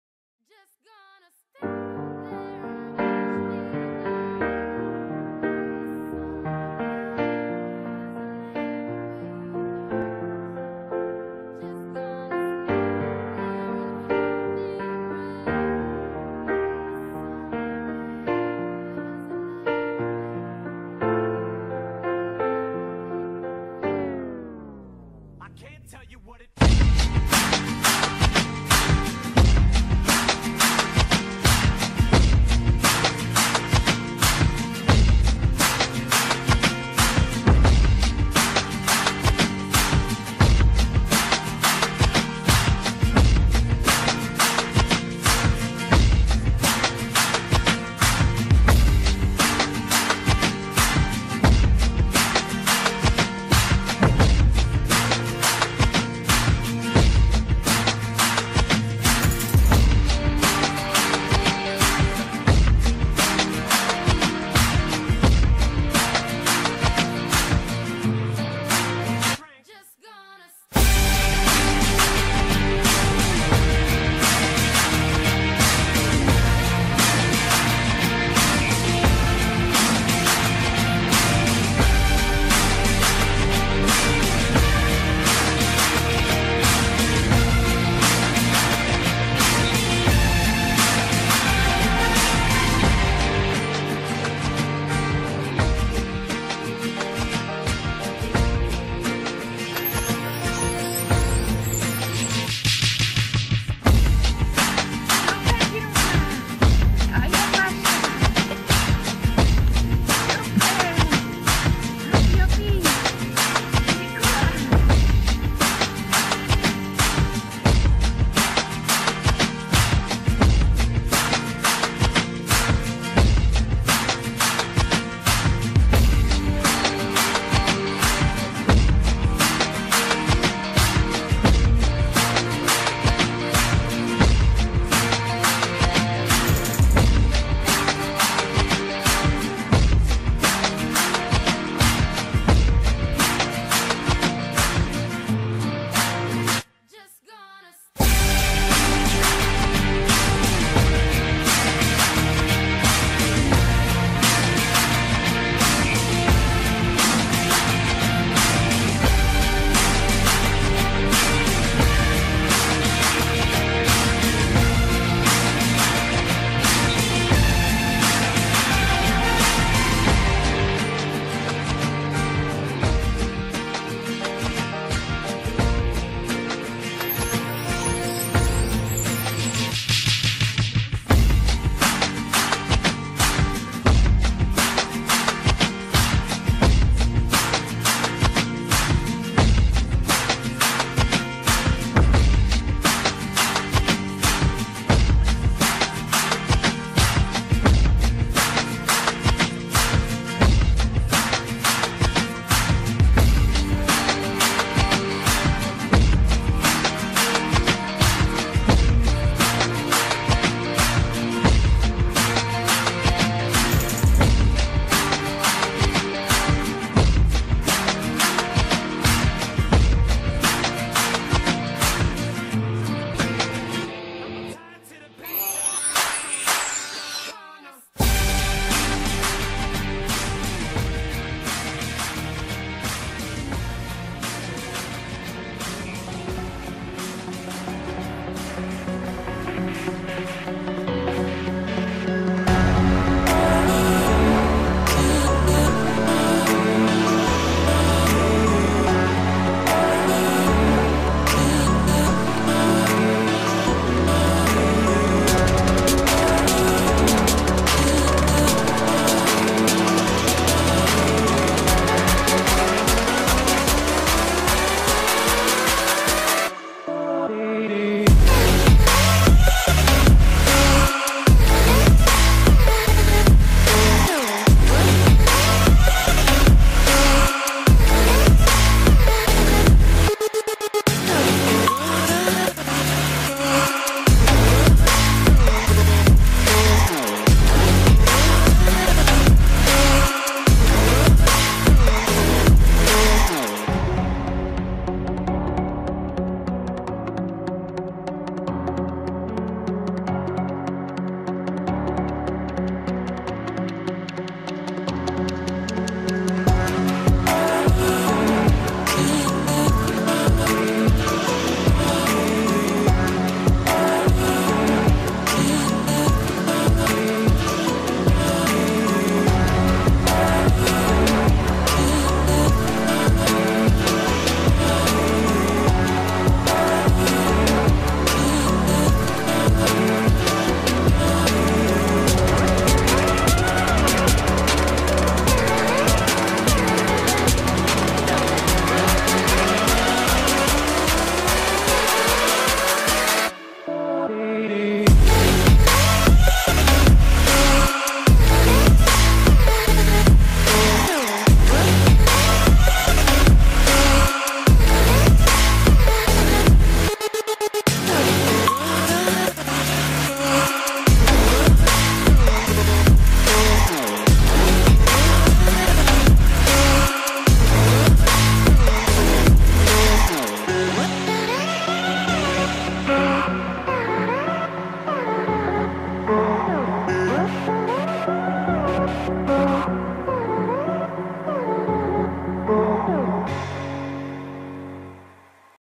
Trénink Fryšták - Kira a Afrodita - 4.11.2018
liška tahá koloběžku :D